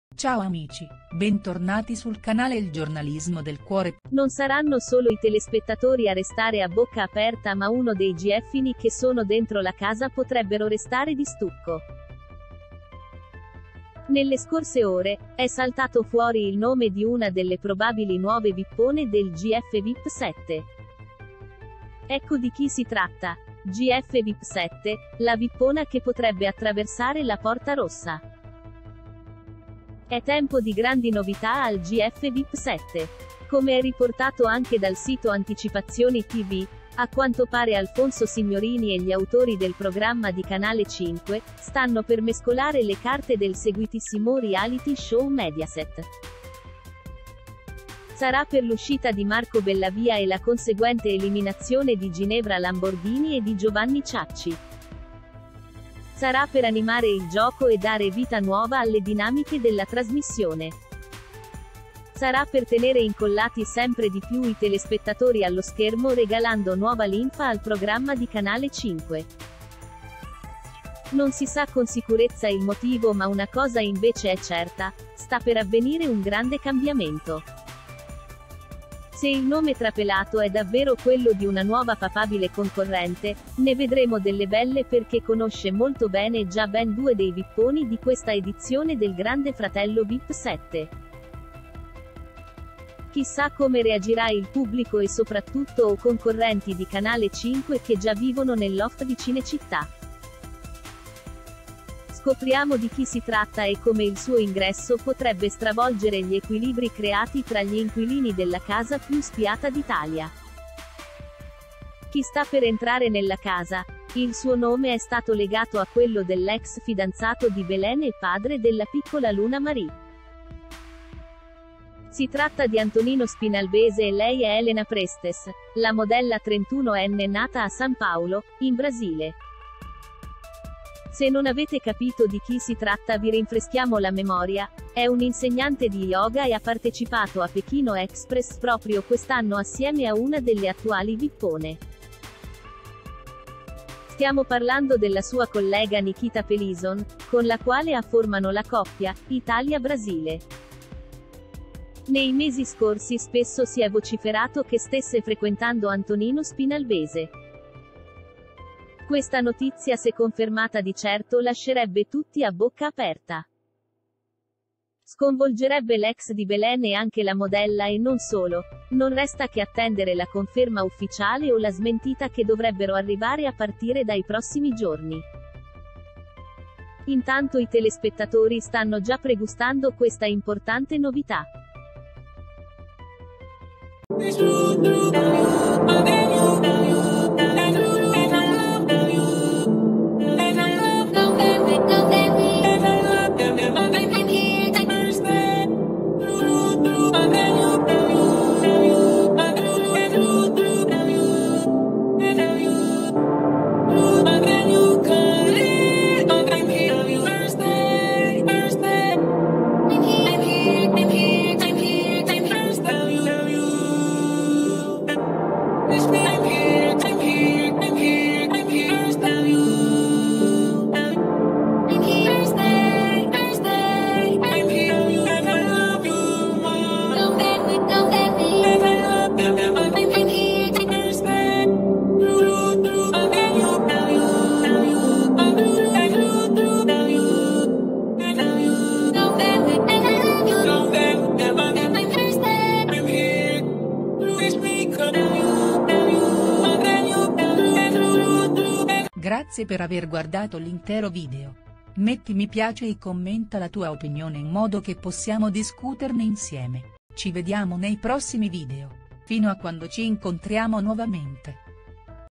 Gf Vip 7, nuova Vippona al reality show chissà come la prenderà Antonino Spinalbese
Incredibilmente al Gf Vip 7 potrebbe ben presto arrivare una nuova concorrente e il pubblico sentendo di chi si tratta di certo strabuzzerà gli occhi. Scopriamo chi è il personaggio misterioso.